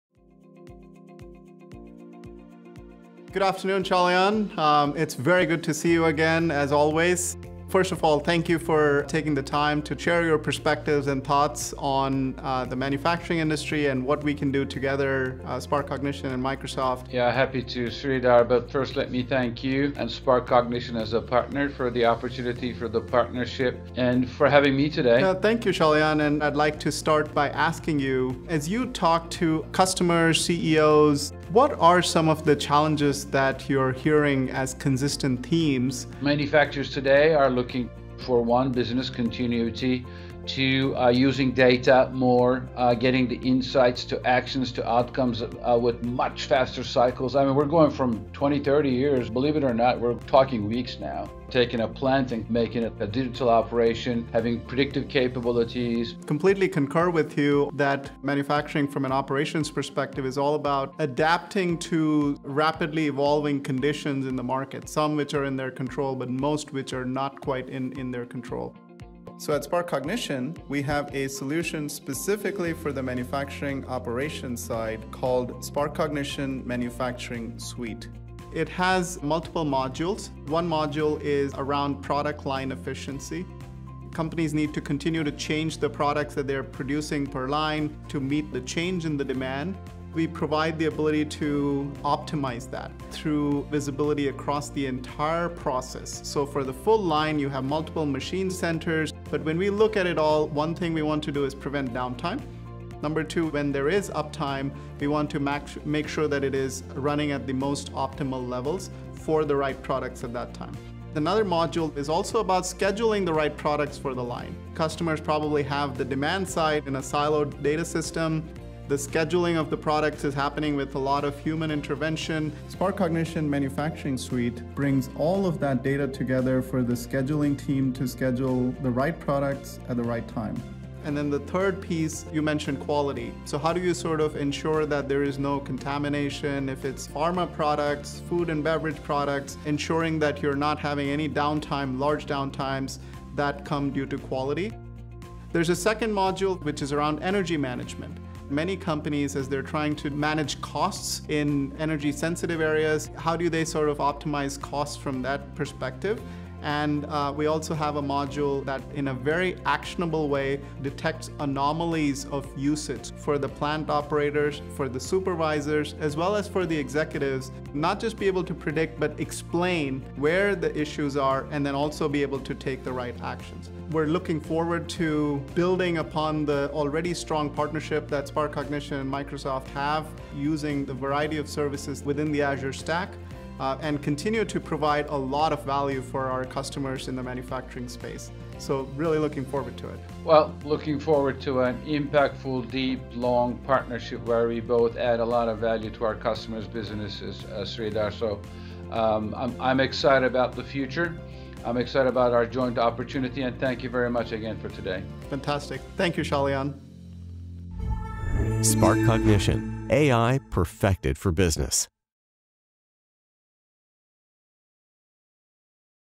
Achieve operational excellence with AI-powered APM for manufacturing
Learn how SparkCognition™ Manufacturing Suite enables operational excellence with end-to-end visibility, predictive maintenance, and prescriptive insights. for more information.